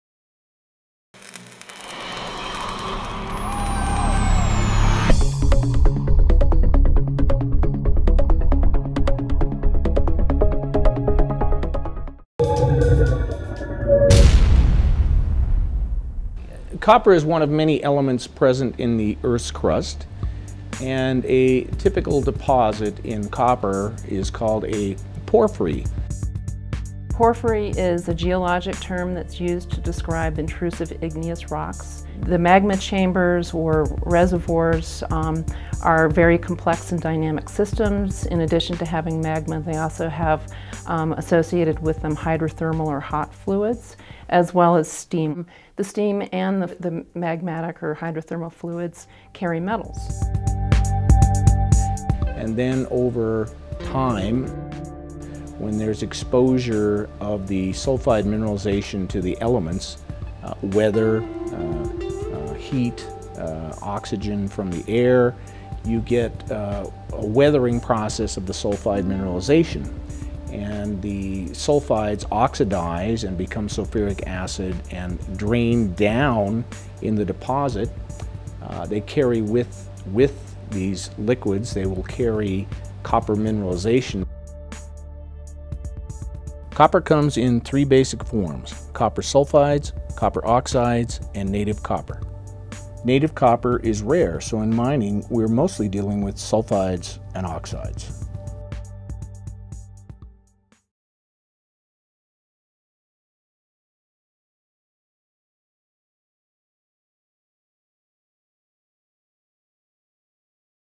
Copper in our Electrical World - Part 2: The Geology of Copper
Learn about how copper deposits form in the earth’s crust. Copper mainly comes in the form of copper sulfides, native copper, and copper oxides. How copper is mined and processed depends on what type of copper mineral you have.